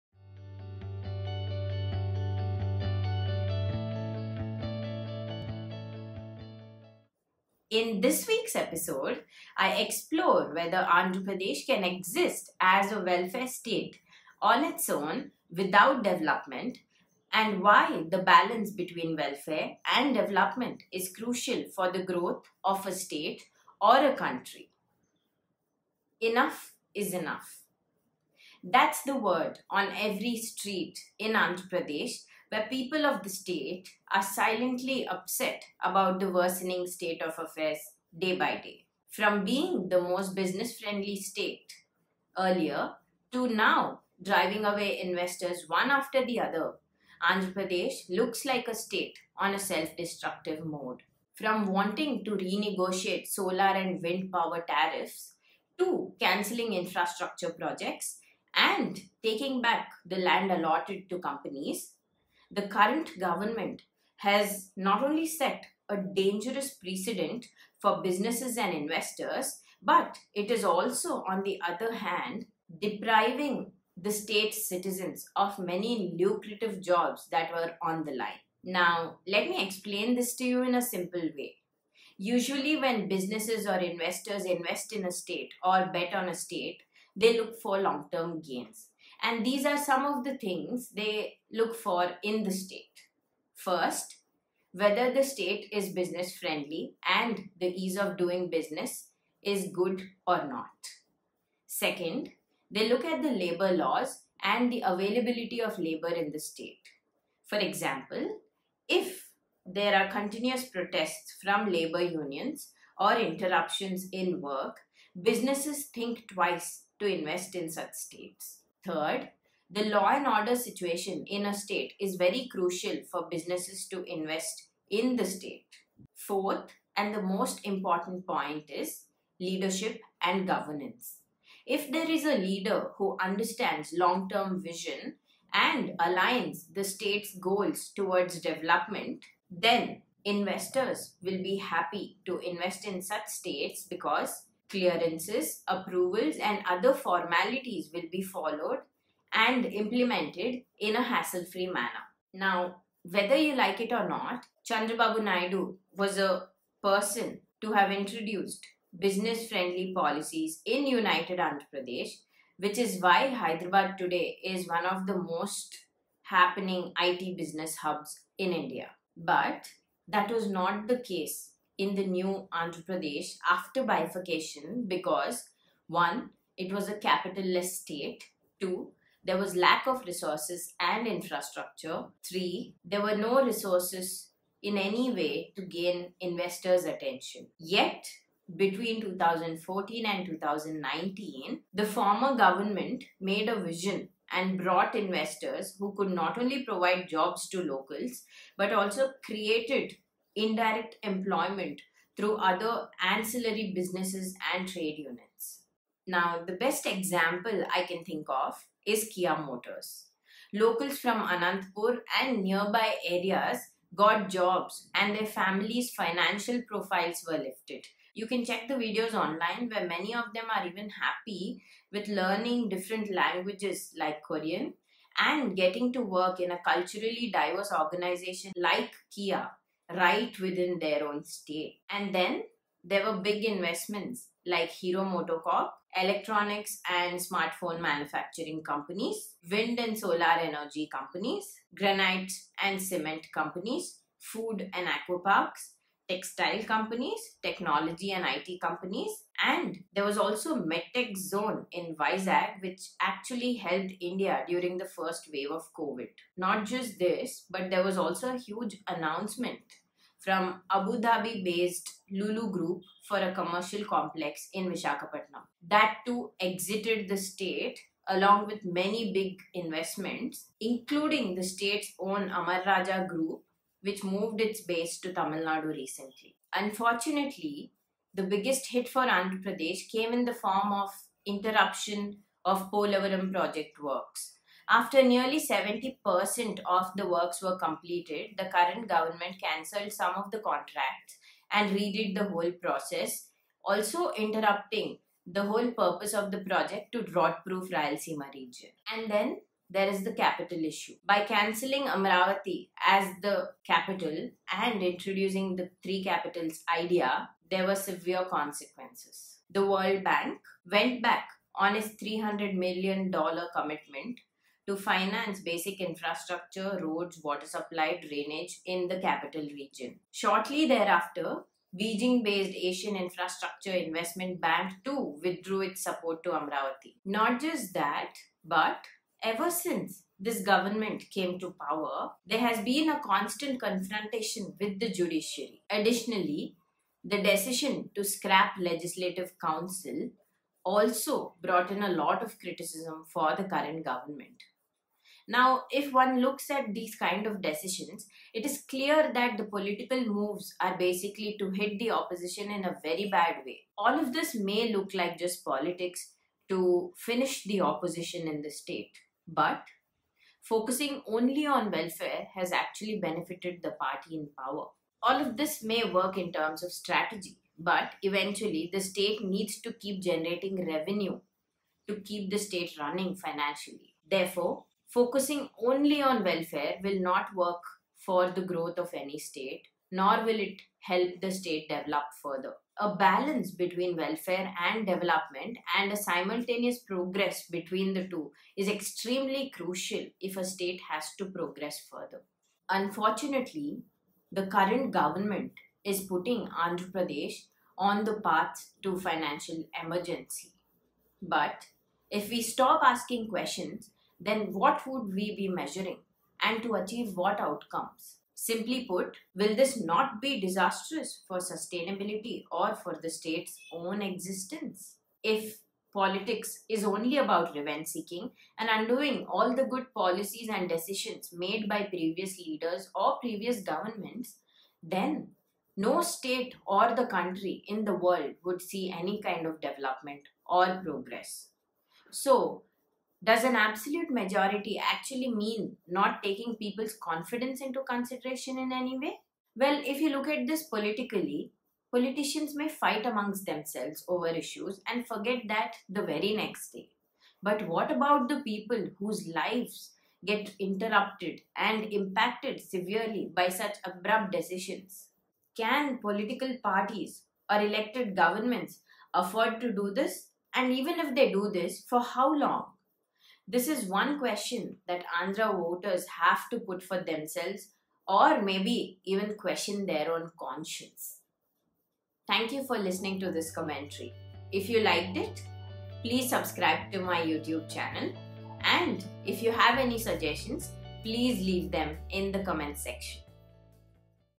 Why Andhra is on the path to financial emergency
Commentary on why welfare politics may not be sustainable for Andhra Pradesh and why it is on the path to financial emergency.